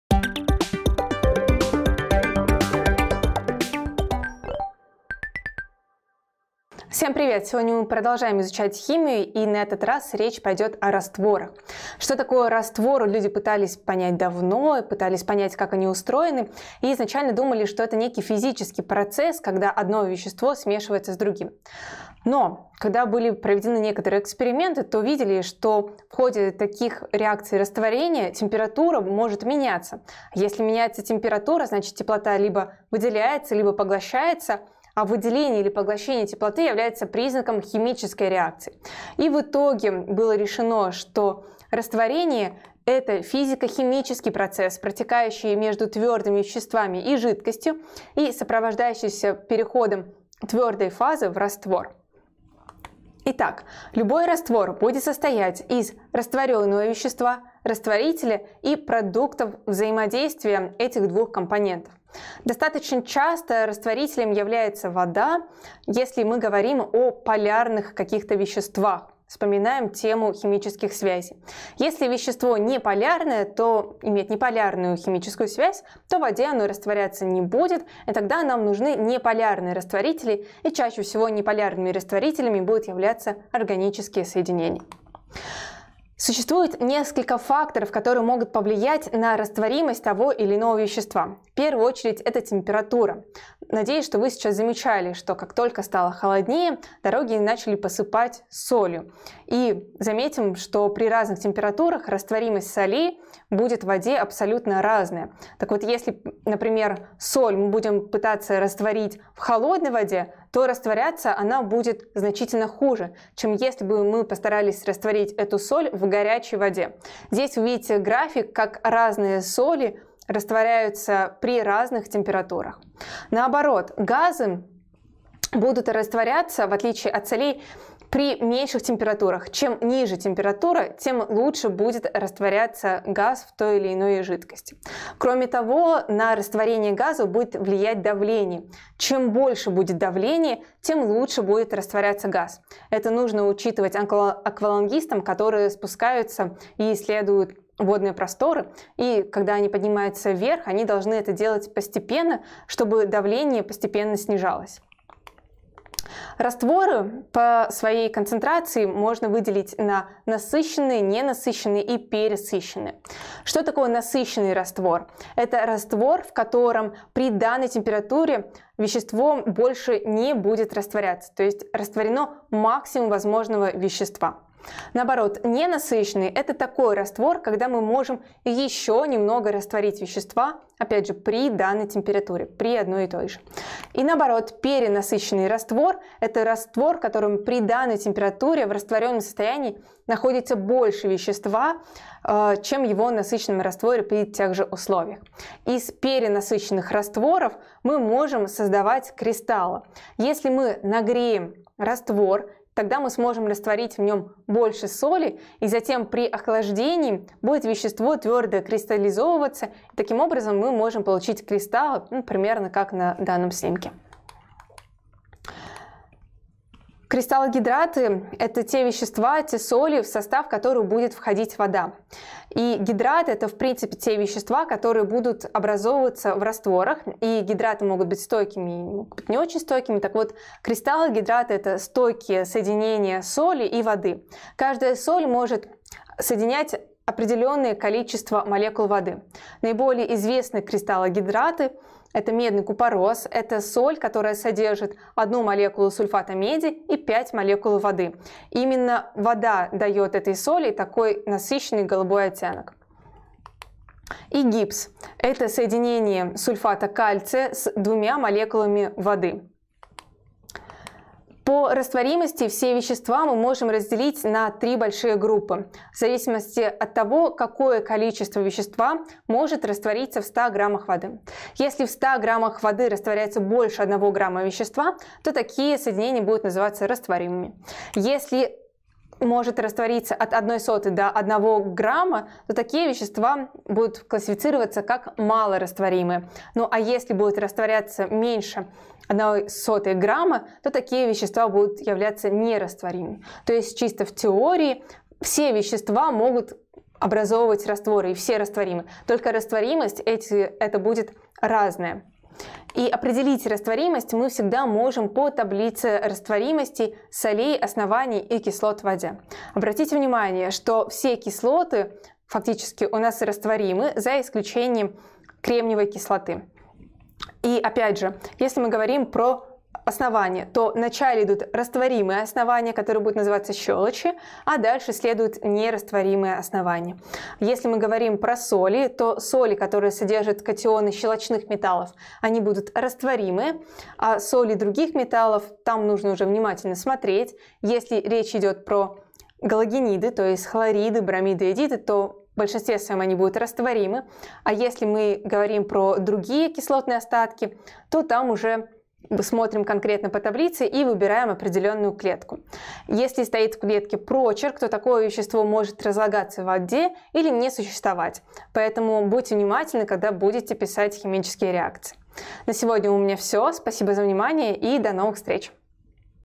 Растворение. Растворимость веществ в воде. Урок 19. Химия 8 класс
Сегодня мы рассмотрим процесс растворения – физико-химический процесс, протекающий между твердыми веществами и жидкостью, сопровождающийся переходом твердой фазы в раствор.

📌Содержание:
0:00 Вступление
0:40 Что такое растворение?
0:53 Из чего состоит раствор?
1:25 Факторы, влияющие на растворимость
2:35 Виды растворов по концентрации
2:43 Насыщенный раствор
2:55 Ненасыщенный раствор
3:04 Перенасыщенный раствор
3:34 Кристаллогидраты
4:20 Вещества по растворимости в воде
5:02 Таблица растворимости кислот, оснований в воде